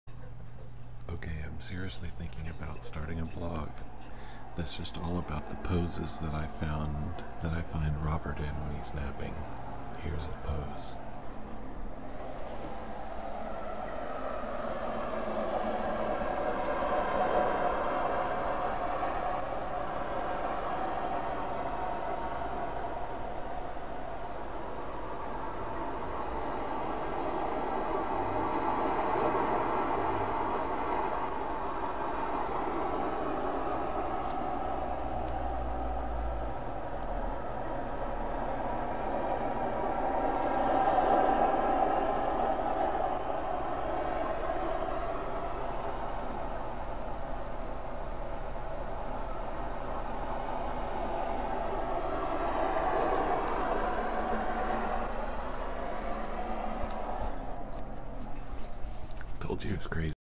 LittleRFL's NapTime Poses #1
LittleRFL does a lot of movement when he sleeps - and he sometimes falls asleep so fast, he's in the middle of doing something - this pose looks like he was just shot and fell back with his legs in the air ... quite a character!